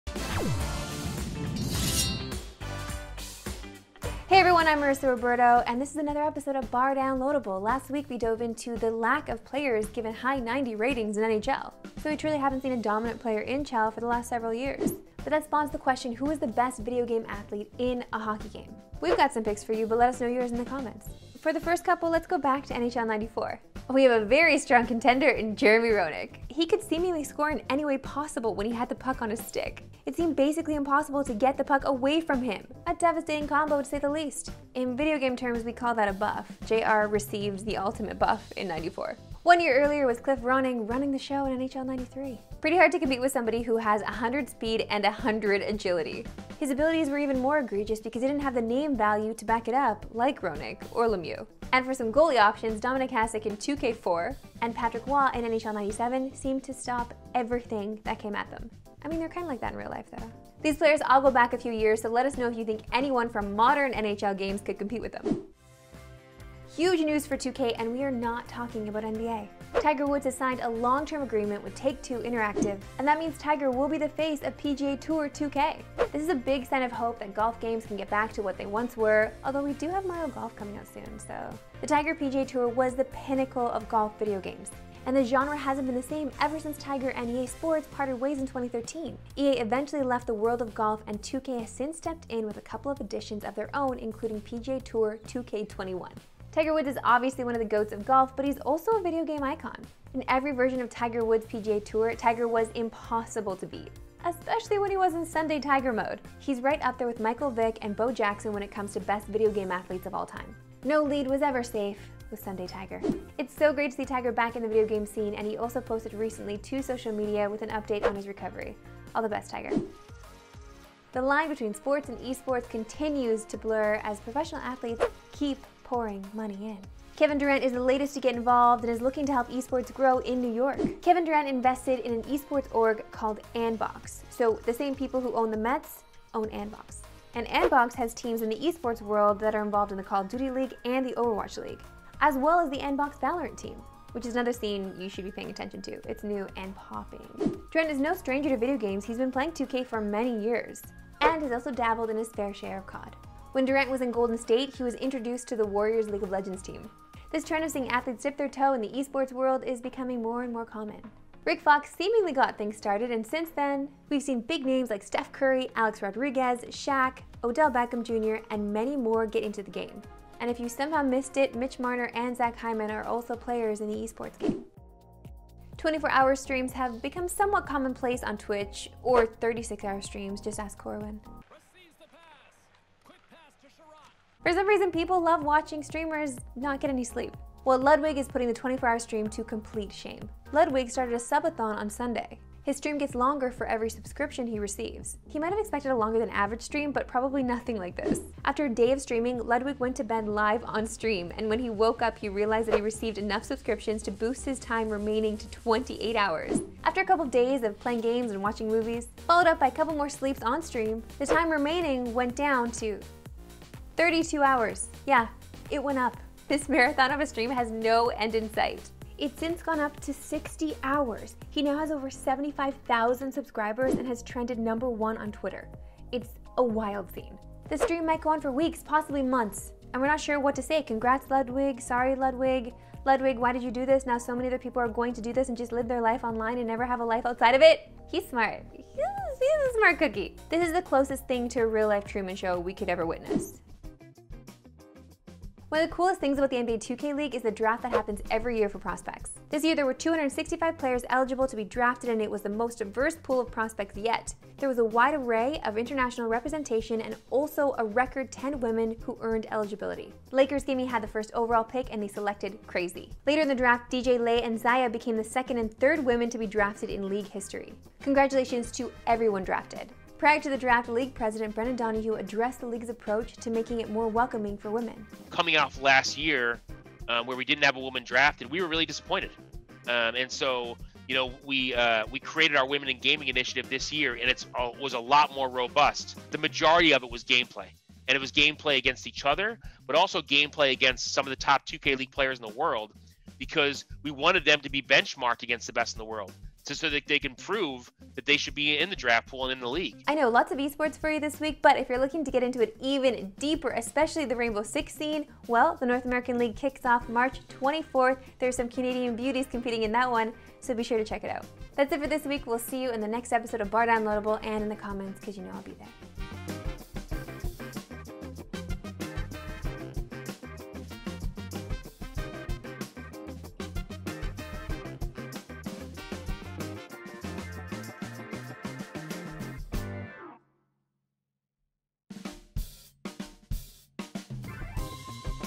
Is Jeremy Roenick the G.O.A.T. of Hockey Video Games?
Is Jeremy Roenick the G.O.A.T when it comes to hockey video games? Marissa Roberto outlines some contenders, and also dives into Tiger Woods joining forces with 2K, as well as, Ludwig's insane never-ending livestream.